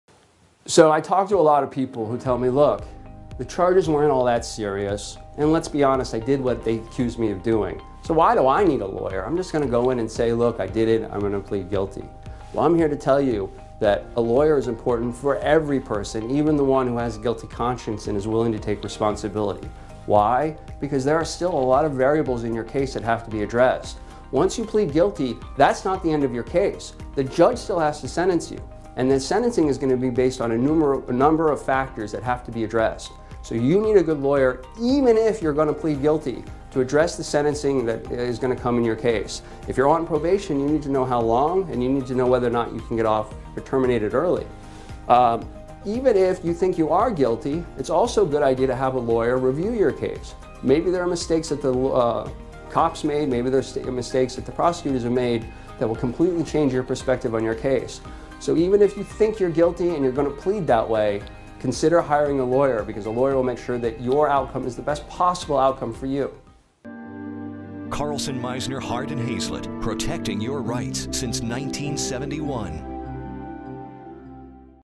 Do You Need A Lawyer If You Plan to Plead Guilty?
If you want to PleadGuilty, what’s the point of hiring a Lawyer? 🤔 Reasonable question.

The thing is, once you plead guilty...that's not the end.😳  Next comes sentencing and there's a number of factors worth considering to help you achieve the best possible outcome! ✊